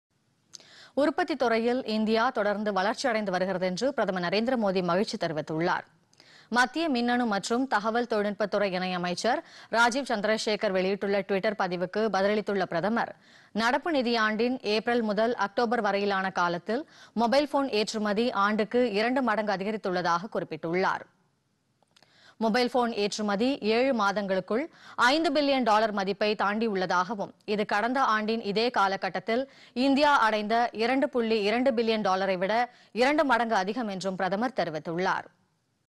மக்களுக்கு பிரதமர் அழைப்பு
உற்பத்தித்துறையில் இந்தியா தொடர்ந்து வளர்ச்சி அடைந்து வருகிறது - பிரதமர் நரேந்திர மோதி பெருமிதம்.